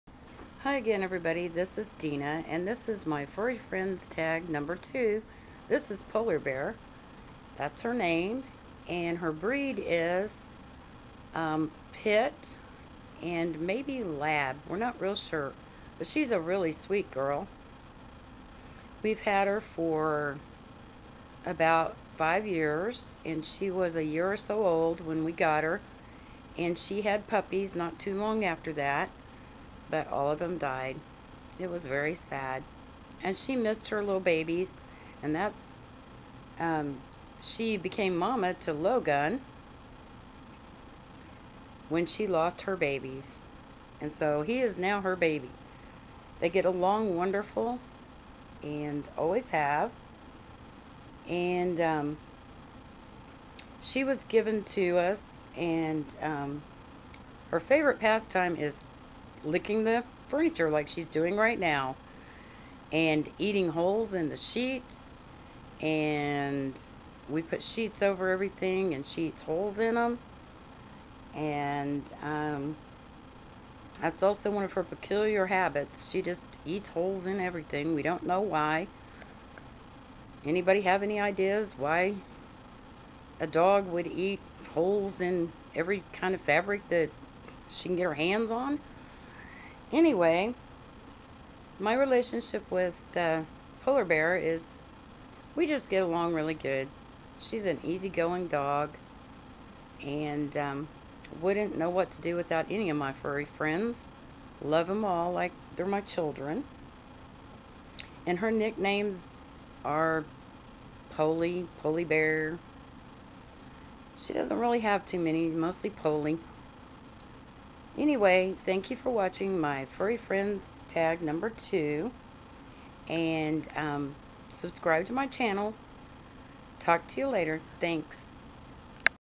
furry friends tag # 2 (Polar Bear)
Furry Friend Tag
Name:  Polar Bear
Breed:  Pit/ lab ?
How long you've had them?  about 5 years she was a year or so old when we got her.
How you got them? She was given to us.
Favorite past time with pet? or in her case her favorite past time is eating anything fabric, she is silly. we take naps together.
What does their relationship mean to you?  like all of them they are my babies.
What peculiar habits do they have?  in polar's case its eating holes in fabric, shes been known to eat zippers out of clothes. very strange.
Nicknames?  polie, pretty girl.
(post pics if you have them)

I watched never 2late4beauty on her channel and she tagged everyone who watched, so now I am tagging all of you. if you have done this tag already, please tag me I'd like to see.
Sorry the lighting and camera aren't that great. I had to go to them, they wouldn't come in where better camera is, not when I want them to. ha,ha.